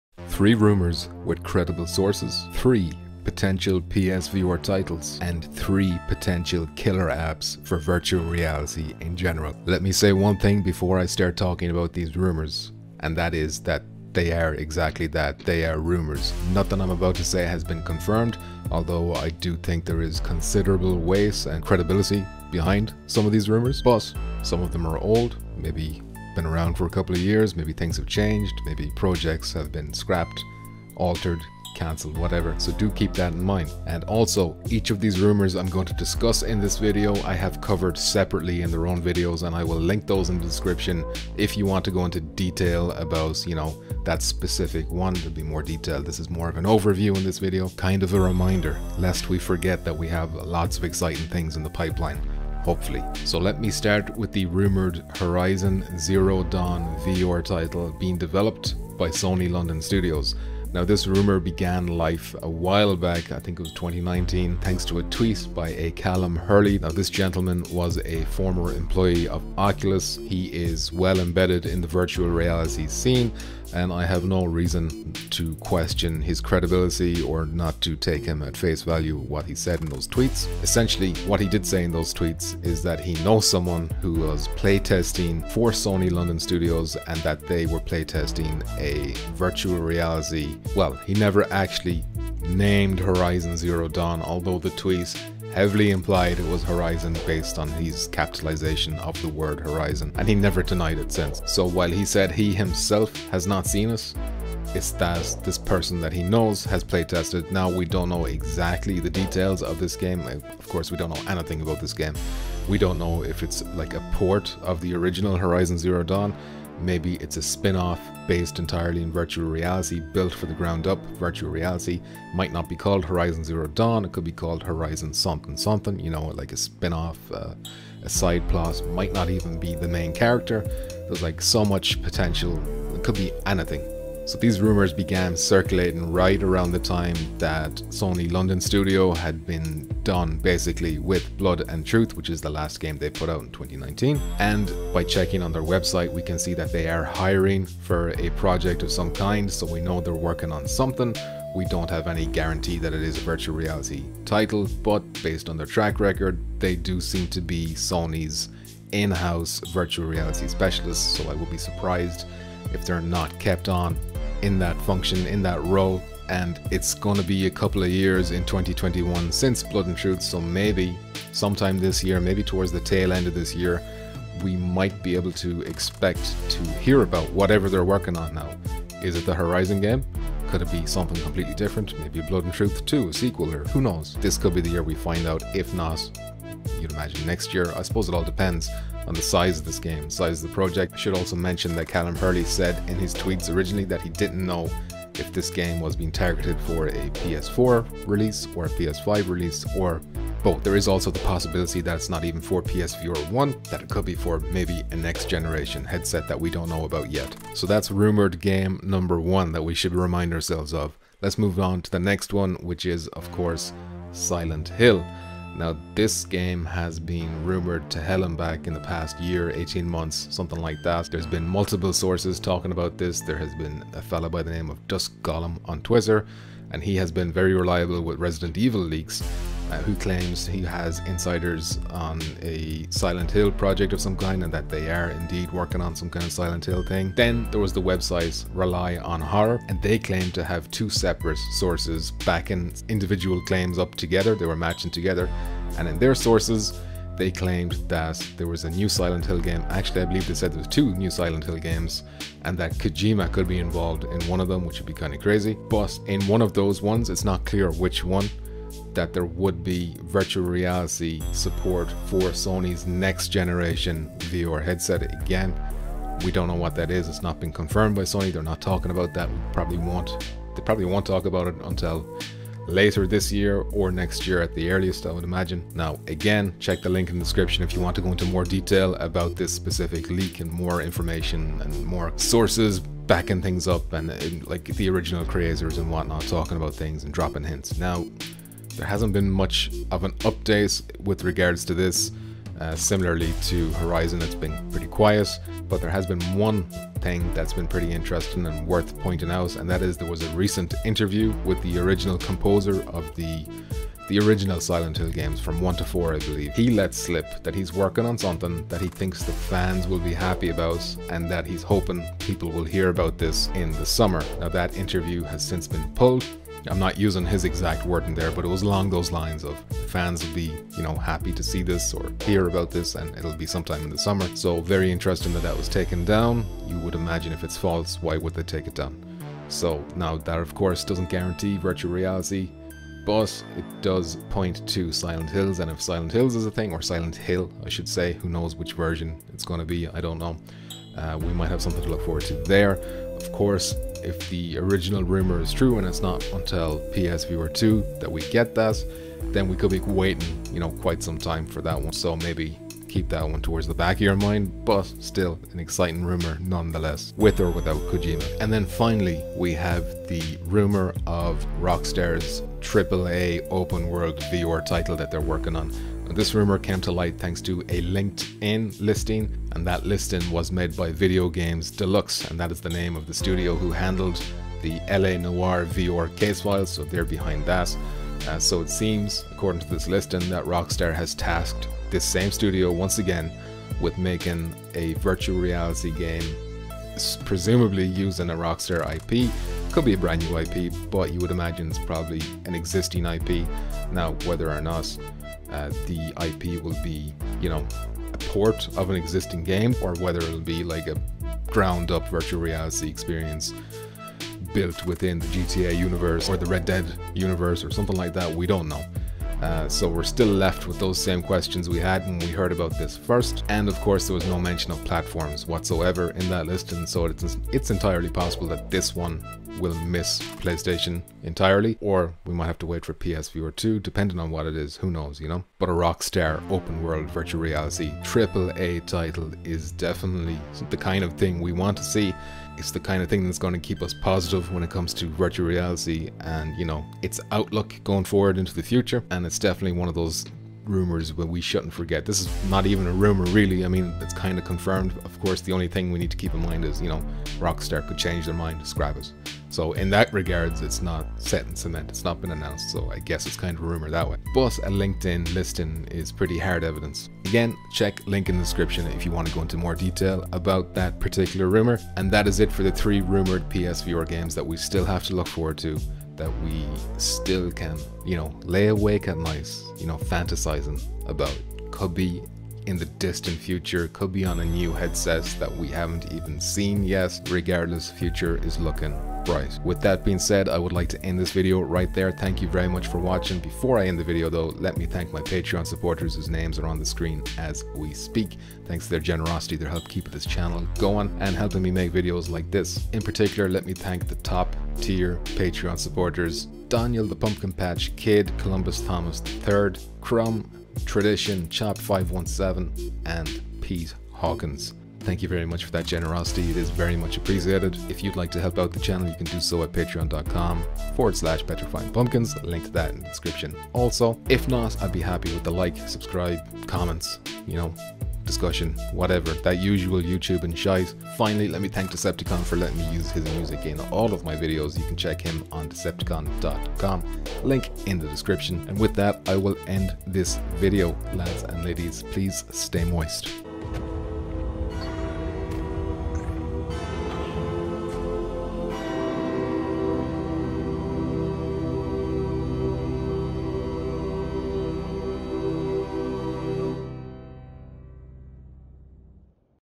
3 BIG PSVR Rumours That You Might Have Forgot
The PSVR has had some exciting rumours in it's life time, some have already come to pass, but don't sleep on these three!